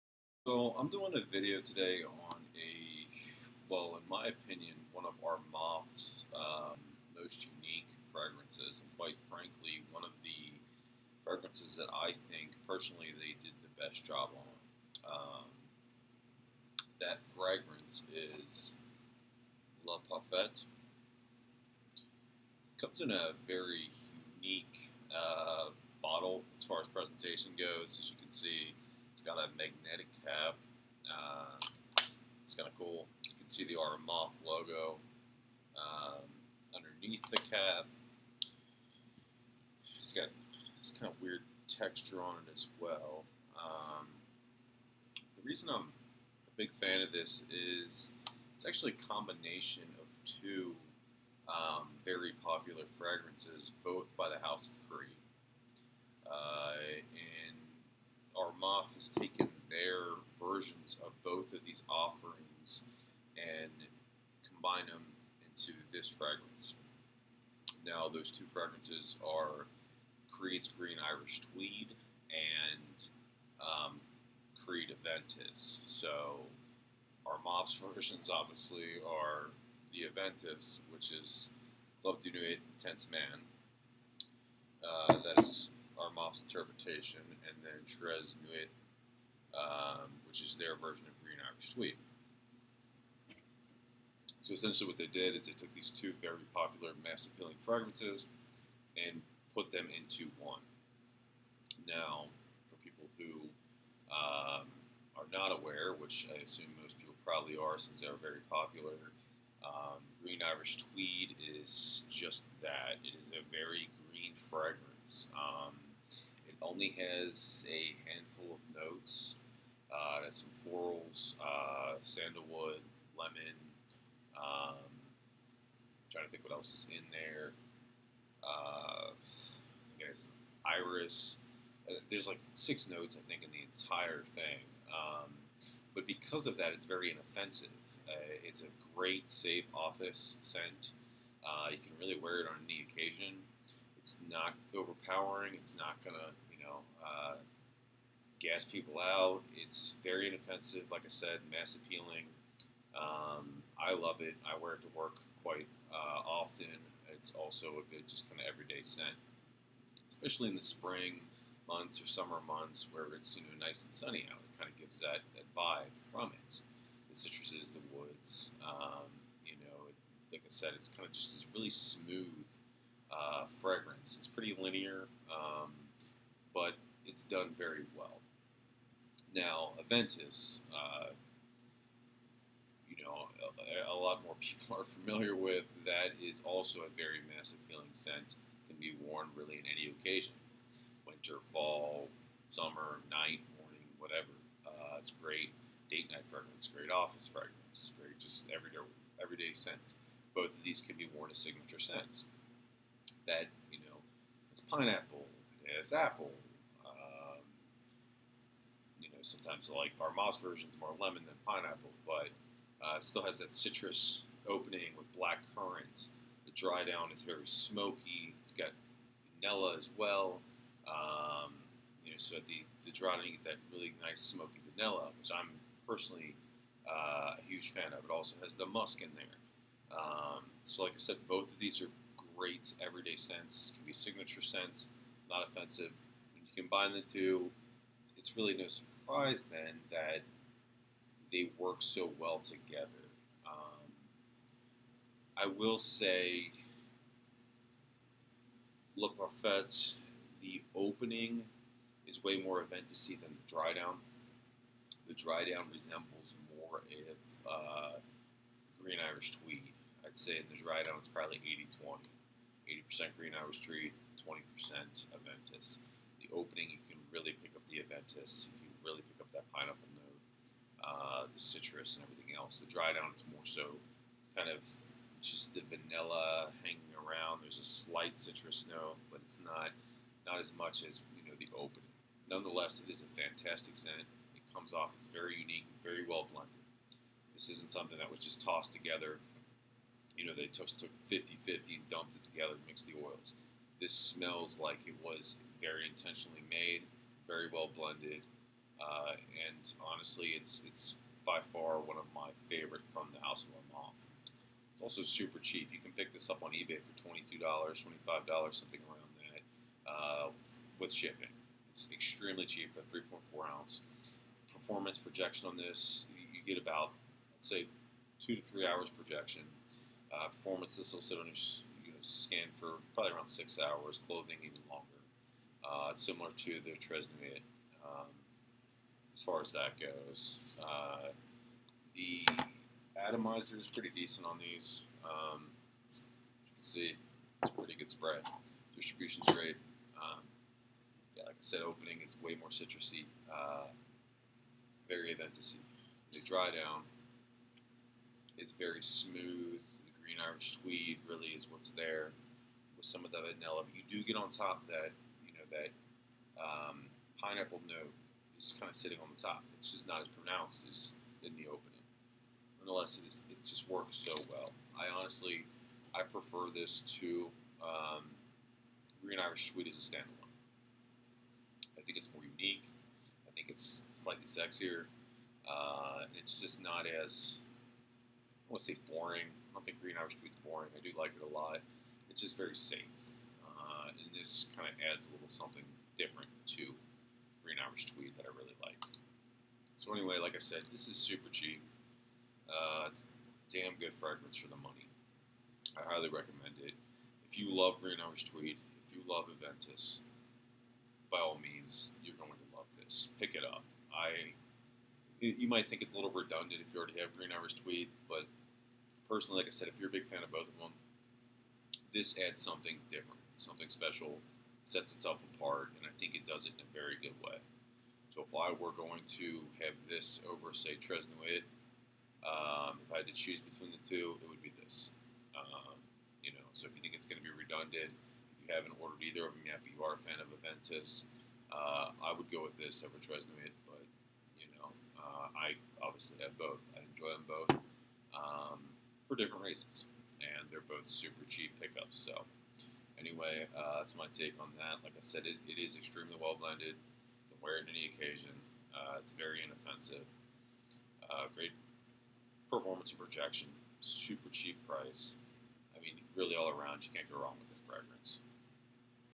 Armaf Le Parfait: The best of Armaf Fragrances?!
In this video I review the house of Armaf's Le Parfrait! A combination of Creed's Aventus and Creed Green Irish Tweed!